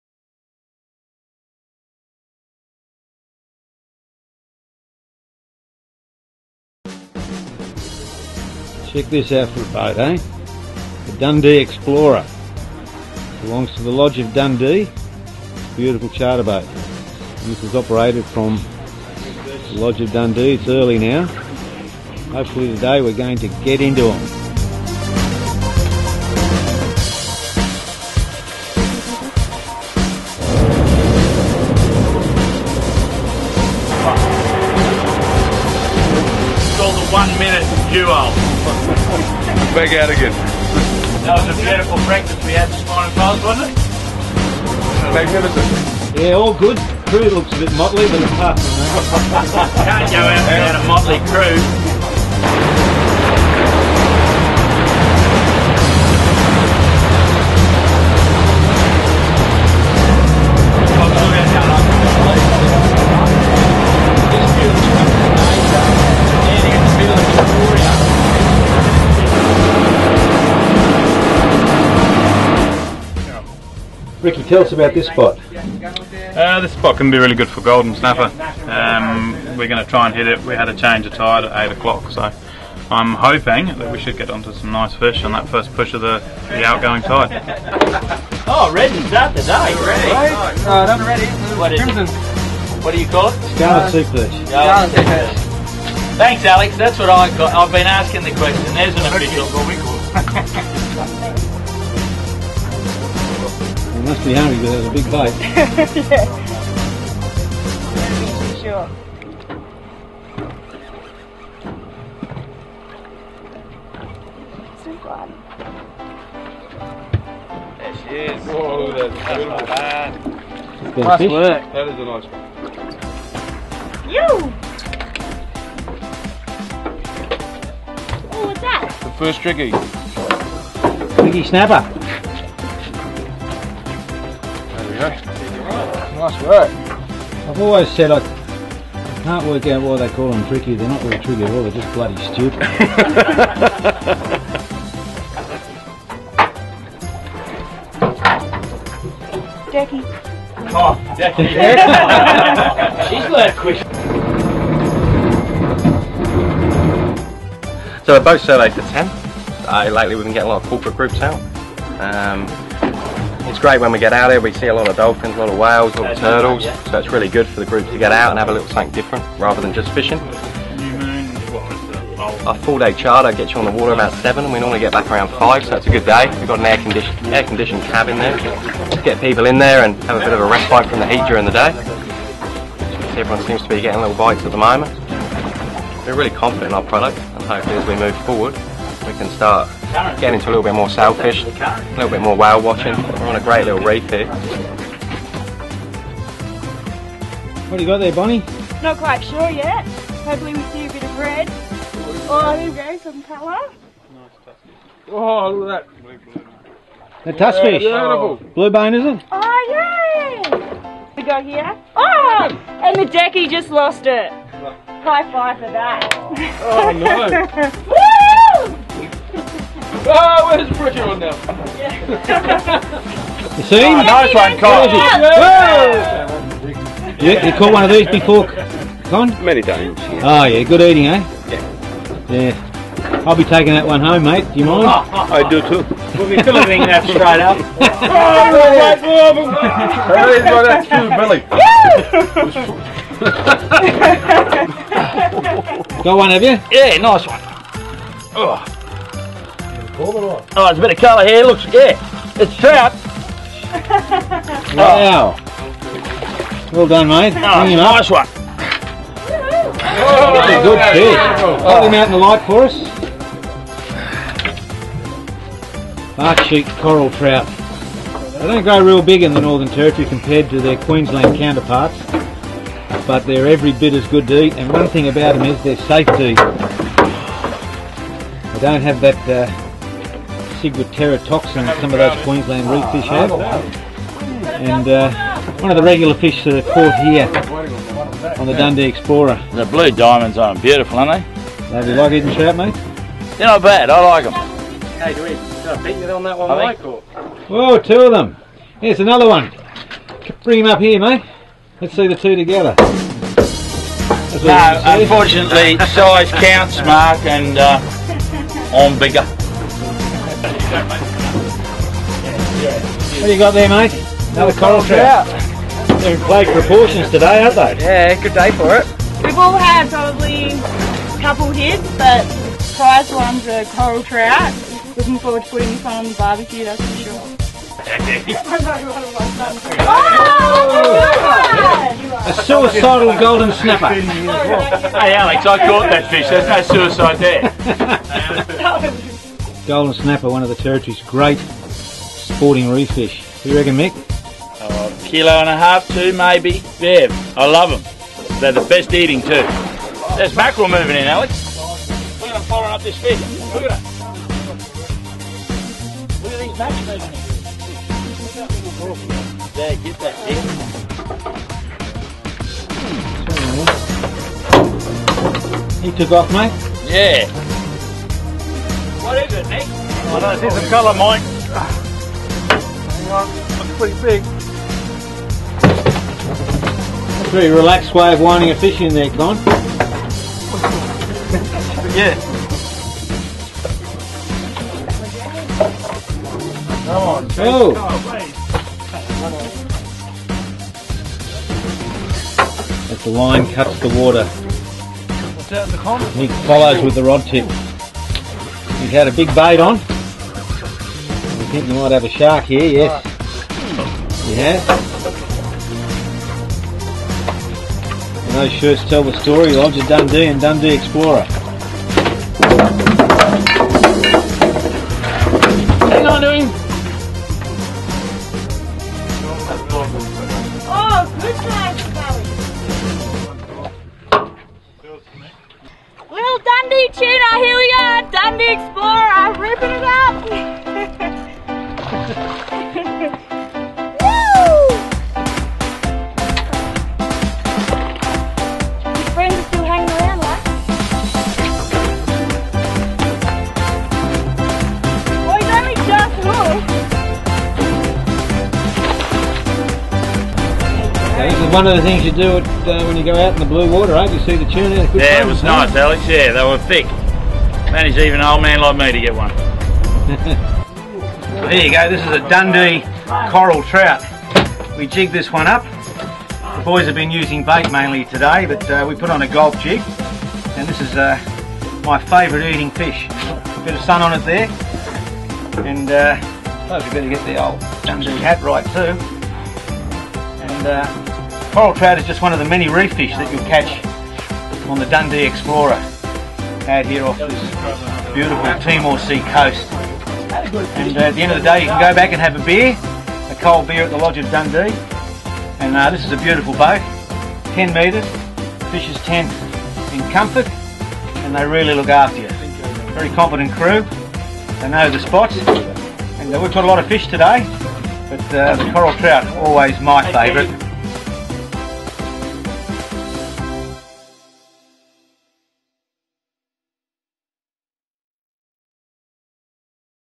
Dundee Explorer – Dundee Beach NT 2014
Enjoy some great reef fishing action aboard the fabulous Dundee Explorer.